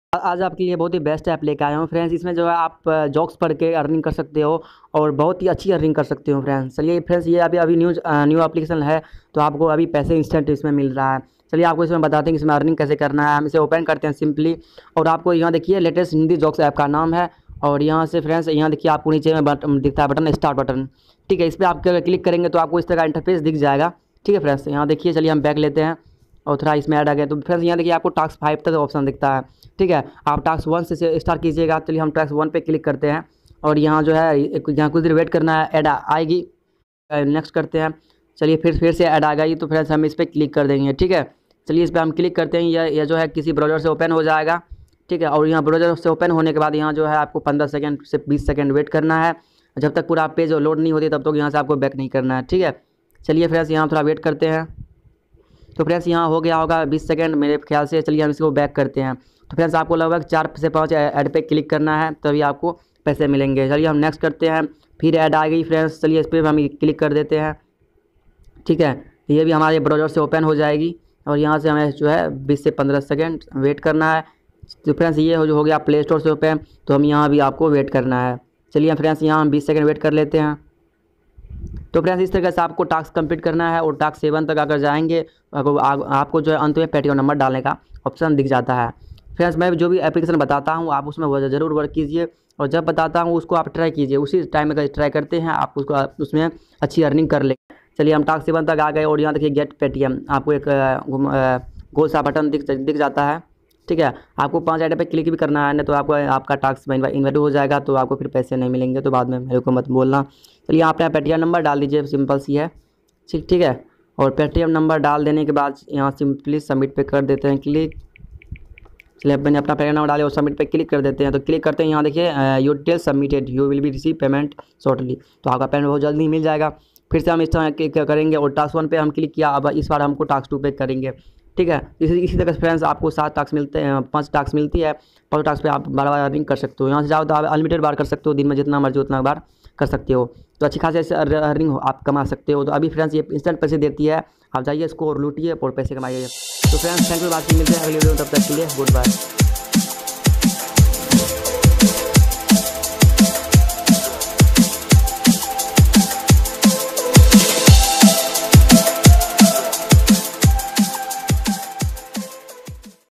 Earn rs1000 Paytm Cash to latest Hindi jokes online earning on Android app
EARN by this app most powerful PayTm cash earn app .
Full plan is this video so wach this video for online money earnining
So THNKYOU

In this video I tell about a new application this application name is Indian coin in this app you can earn unlimited free PayTM cash must watch.

Term and conditions of money guru app_______
1. दोस्तों अगर जल्दी-जल्दी में ऐड पर क्लिक हो जाती है तो ऐड को ओपन होने दें और 20 से 30 सेकंड तक इंतजार करें उनके बाद आप पीछे कर ले

2. दोस्तों आप यहां से अनलिमिटेड बार टैक्स कंप्लीट कर सकते हैं जिससे आपको काफी फायदा होगा और ज्यादा पैसे कमा पाएंगे
3. दोस्तो आप को कम से कम तीन ऐड पर क्लिक करना होगा नहीं तो आपका रिक्वेस्ट invalid हो जाएगा

In this app you can earn money free unlimited paytm cash.